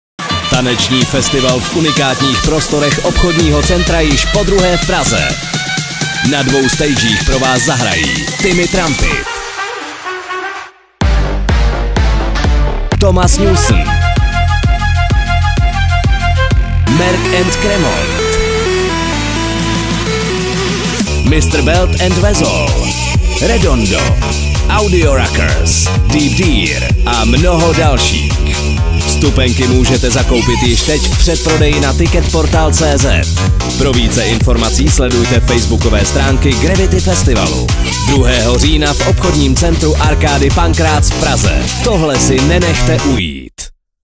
Teaser Gravity festival Arkády Pankrác 2015
Australský DJ a instrumentalista Timmy Trumpet již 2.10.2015 rozezní hlavní stage druhého ročníku Gravity festivalu v obchodním centru Arkády Pankrác. Společně s ním zde vystoupí také Thomas Newson, Merk & Kremont, Redondo a mnoho dalších.